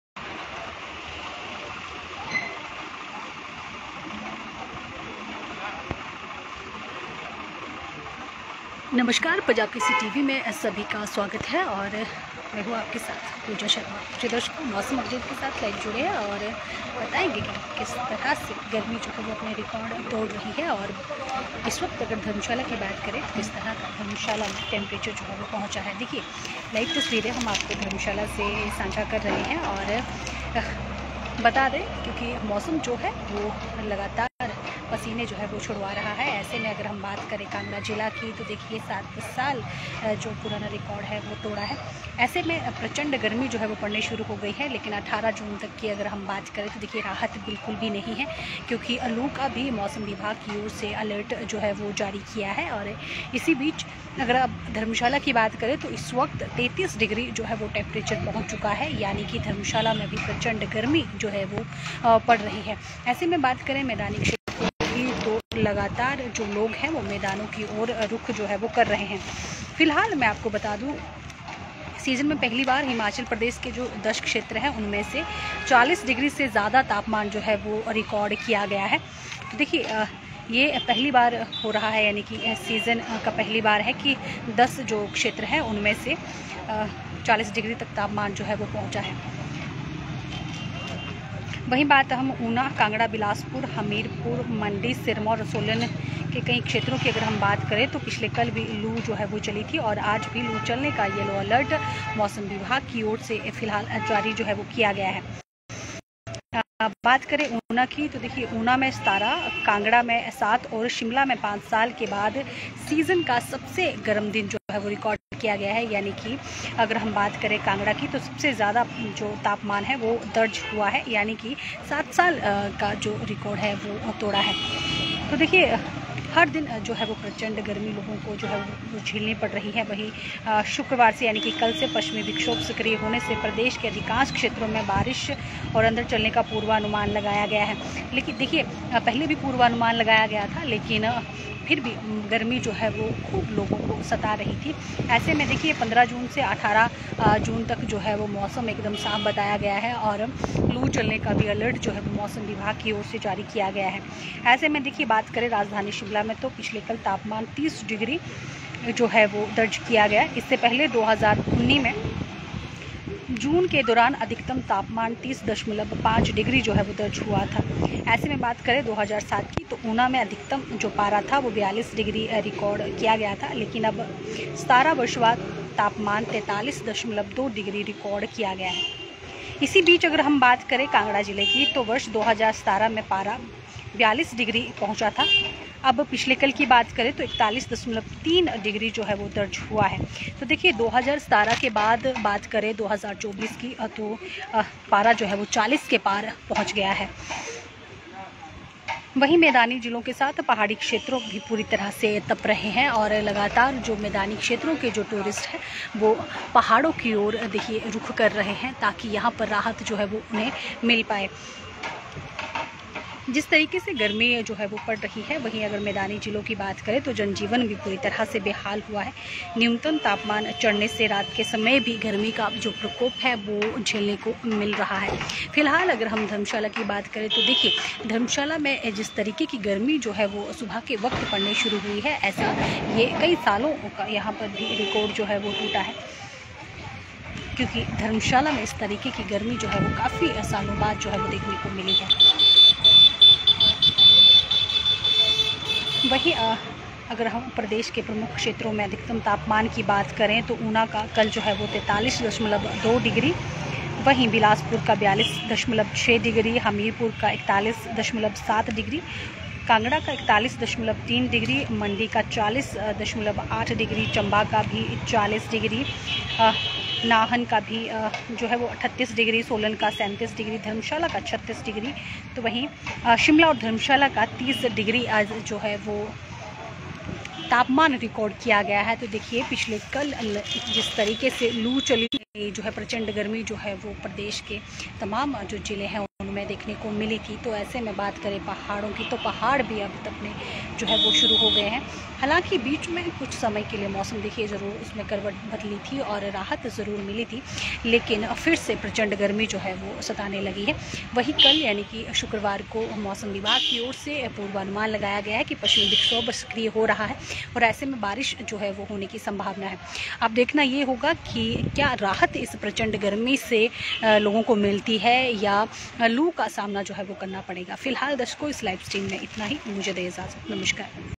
धर्मशाला Live: प्रचंड गर्मी ने कांगड़ा में तोड़ा 7 साल का रिकॉर्ड, गर्मी से 18 तक नहीं कोई राहत
धर्मशाला Live: प्रचंड गर्मी ने कांगड़ा में तोड़ा 7 साल का रिकॉर्ड, गर्मी से 18 तक नहीं कोई राहत, आज भी लू का अलर्ट जारीLive...